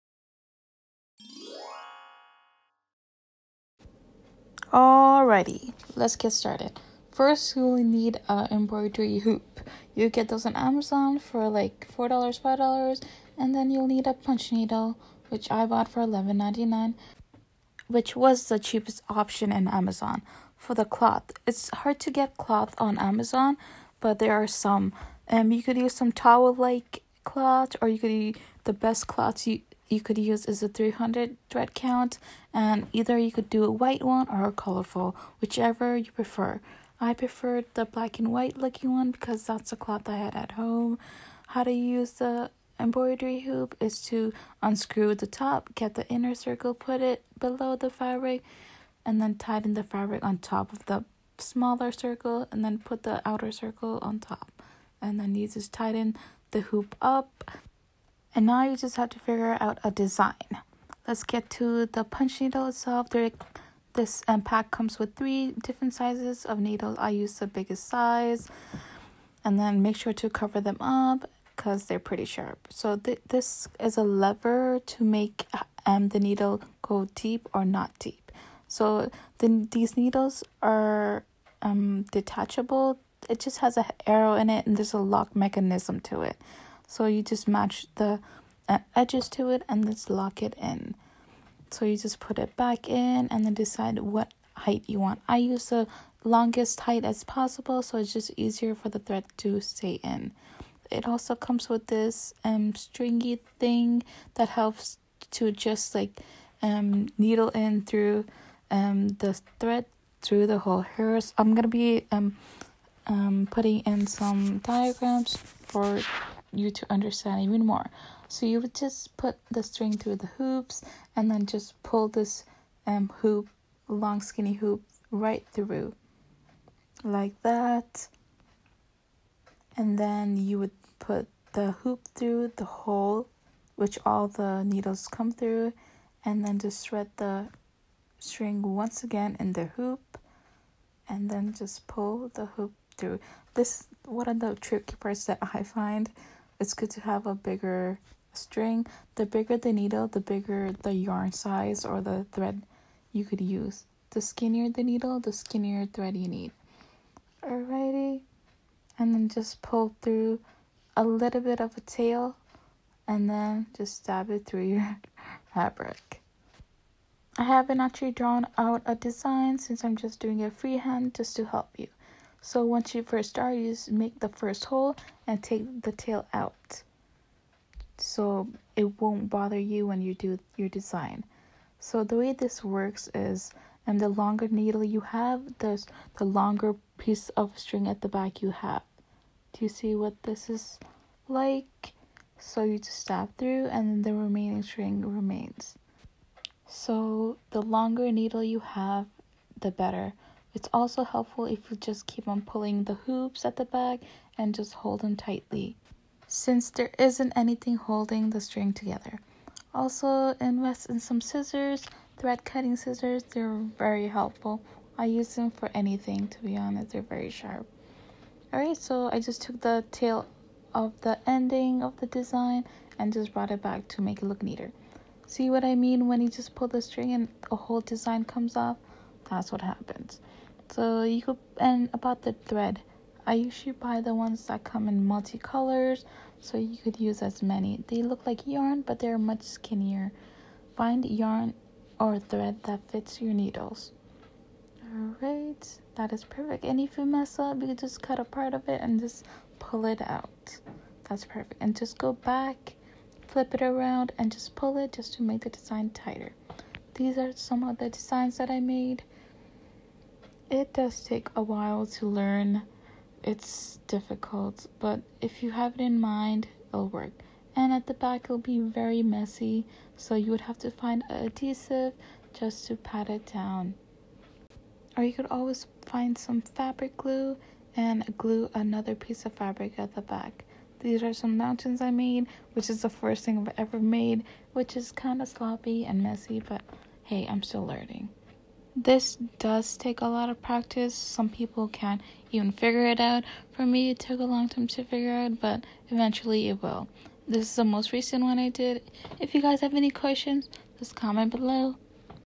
Tutorial: How to use a Punch Needle for Embroidery
This is a Tutorial on How to use a Punch Needle for Embroidery

There aren't many clear videos on the punch needle, so what better way to understand something than to teach.

Ive been addicted to this for so long, and it is a tricky hobby but I alway have to master each thing I pick up and start doing

Hope this video is informative.

If you have any other DIY suggestions i am happy to make tutorials on them.